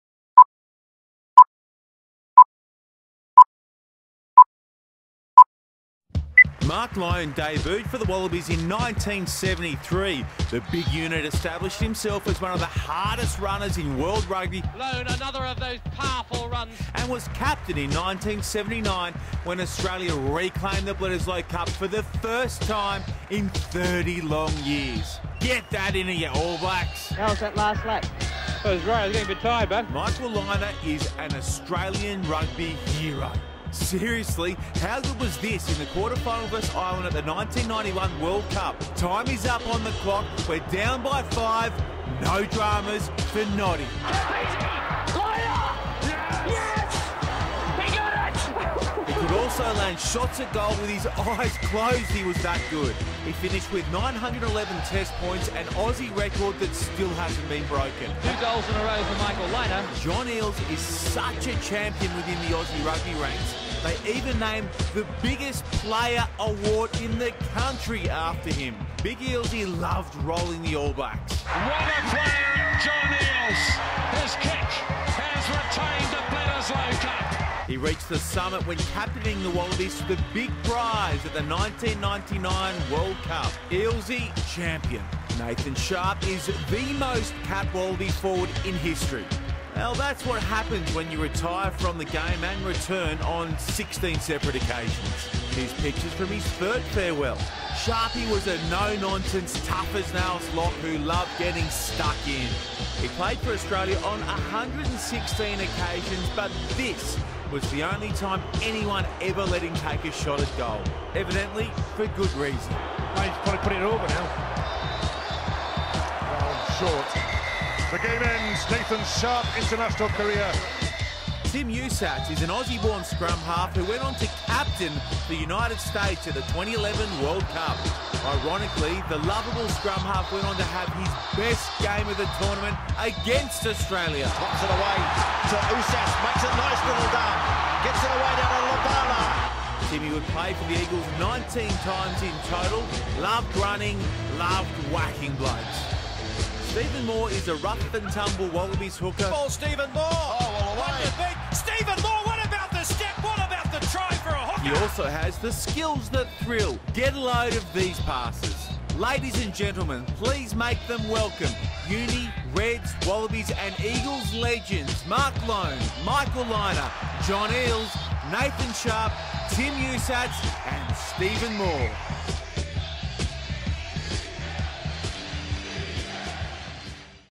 Rugby greats
Rugby greats cheat the Gods making history and cementing their legacy Wallabies All Blacks Nathan Sharpe Mark Loane Stephen Moore John Eales Michael Lynagh Tim Usasz